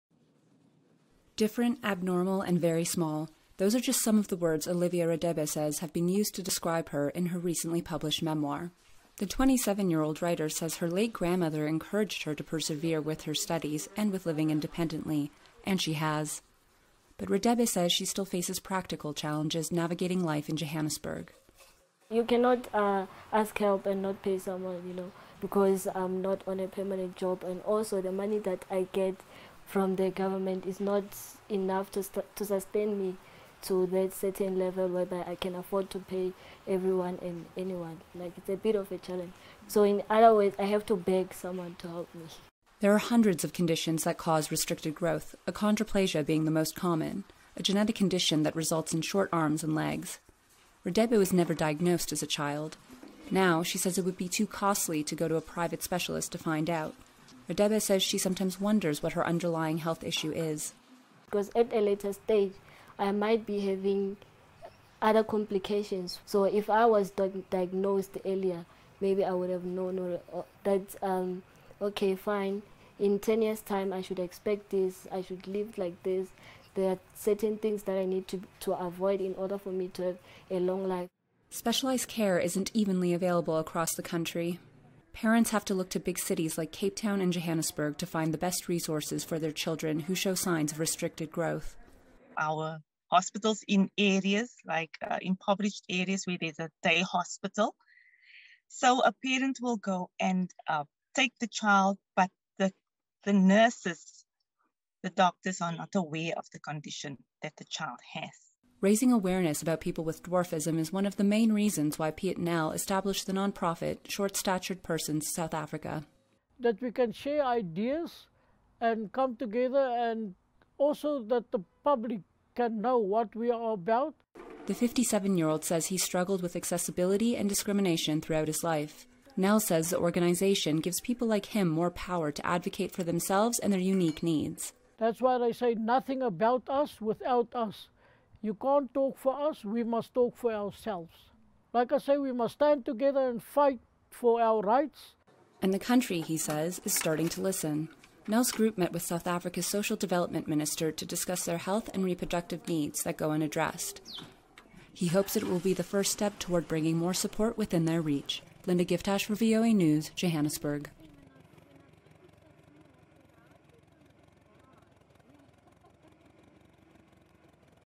South Africa Little People
From navigating shopping centers to government offices, South Africa’s Little People say public spaces aren’t designed to be accessible for them. One advocacy group in Johannesburg is calling for better support to help people live independently.